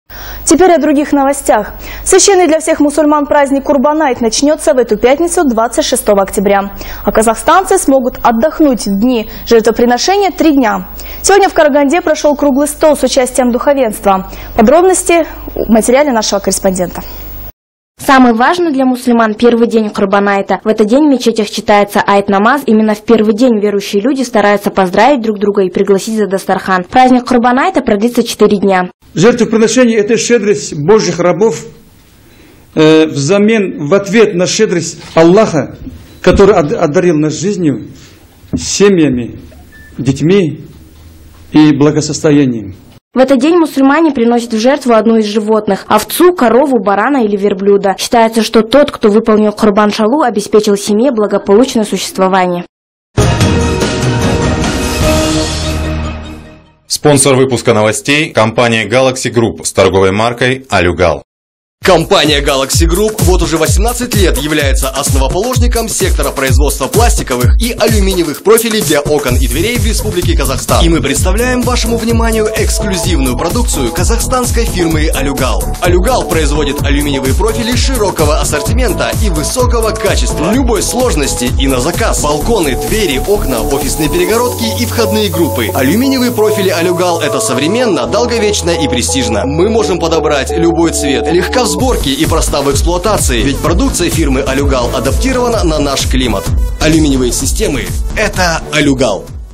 22_10_12_kurban_ait
Священный для всех мусульман праздник Курбан-айт начнется в эту пятницу 26 октября. А казахстанцы смогут отдохнуть в дни жертвоприношения 3 дня. Сегодня в Караганде прошел круглый стол с участием духовенства. Подробности в материале Даны Марат